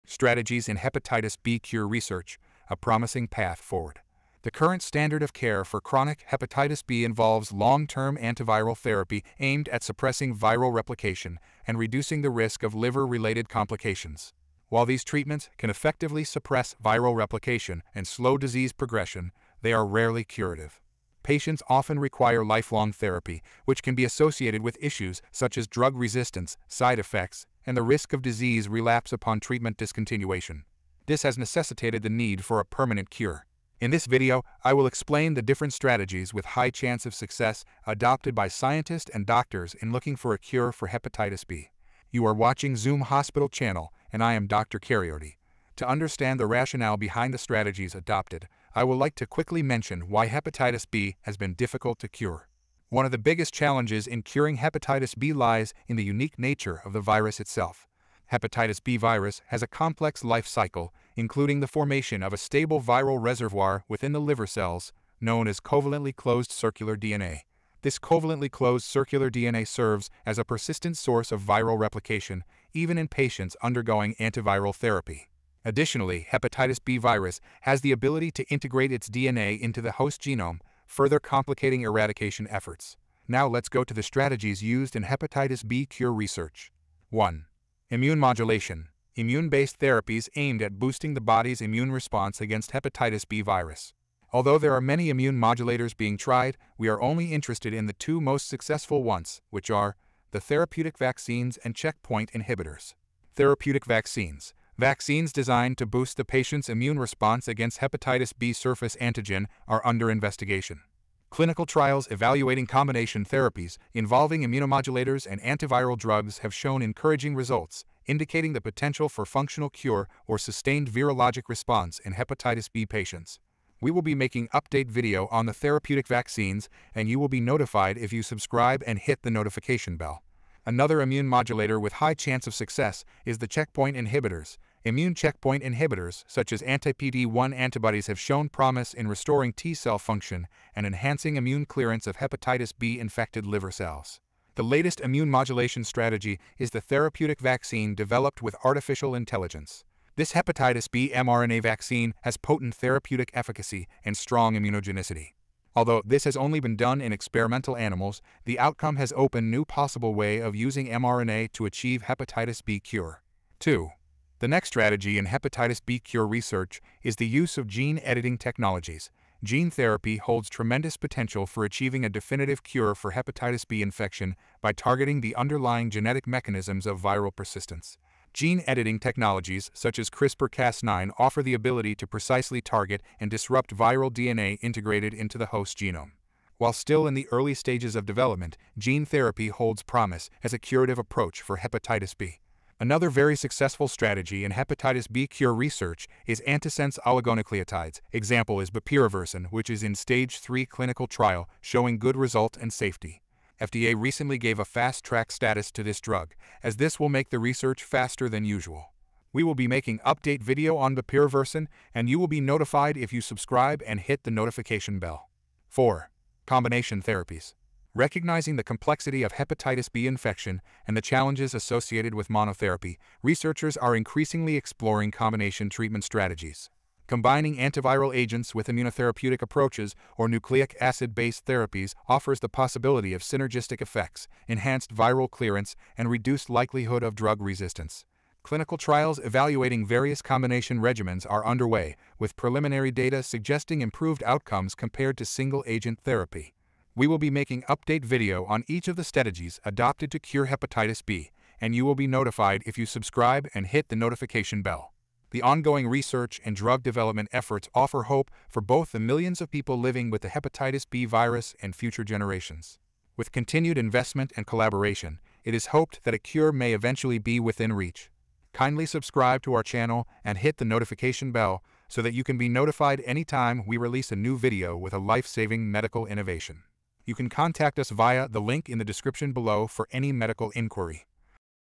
Strategies in Hepatitis B Cure Research | Update on Hepatitis B Cure with A Promising Path Forward
Care for chronic hepatitis B involves long-term antiviral therapy aimed at suppressing viral replication and reducing the risk of liver-related complications. While these treatments, can effectively suppress viral replication and slow disease progression, they are rarely curative. Patients often require lifelong therapy, which can be associated with issues such as drug resistance, side effects, and the risk of disease relapse upon treatment discontinuation. This has necessitated the need for a permanent cure
I will explain the different strategies with high chance of success adopted by scientist and doctors in looking for a cure for hepatitis B
OURWEBSITE;